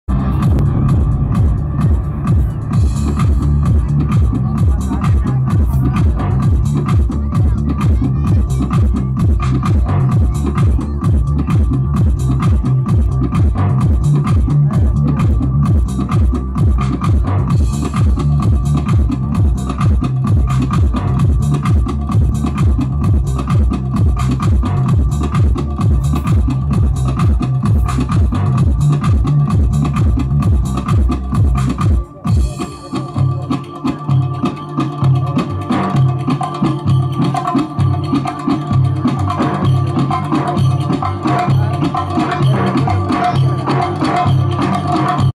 International laser angel - Sanskriti events
International Laser angel from UK performing laser act live.